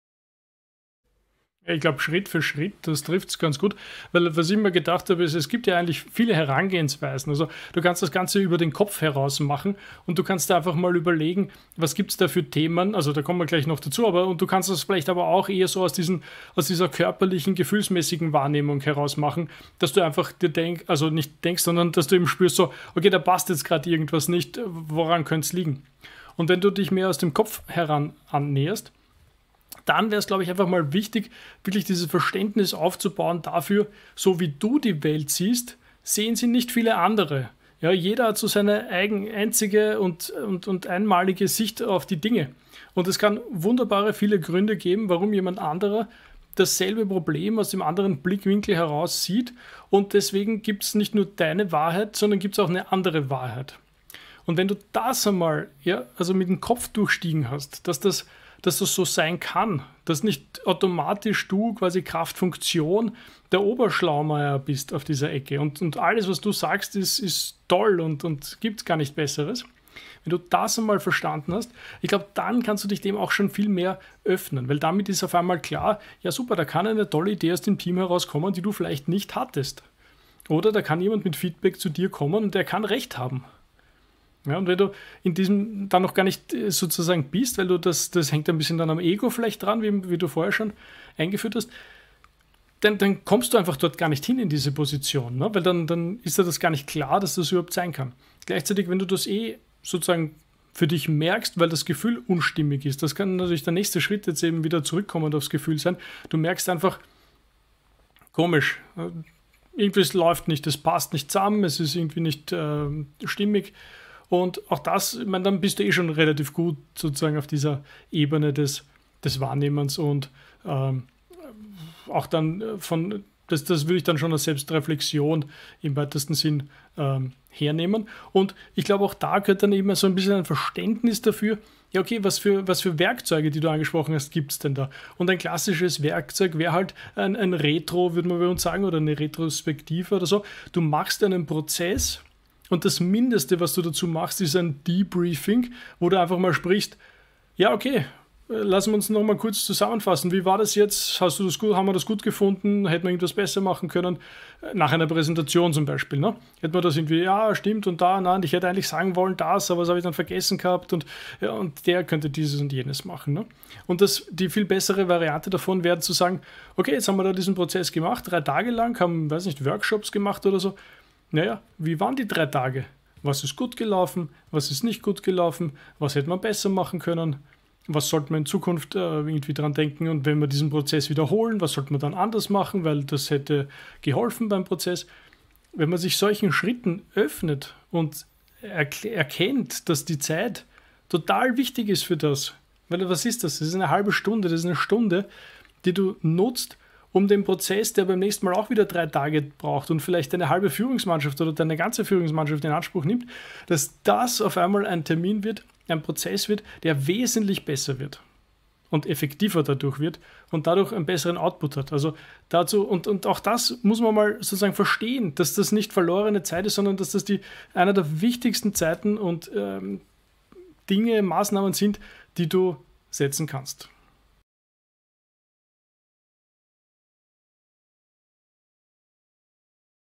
Wie schärfe ich meine Sinne als neue Führungskraft?
In diesem kurzen Clip spricht Flo über die Wichtigkeit einer Reflexion als neue Führungskraft und dem Blick auf andere Perspektiven.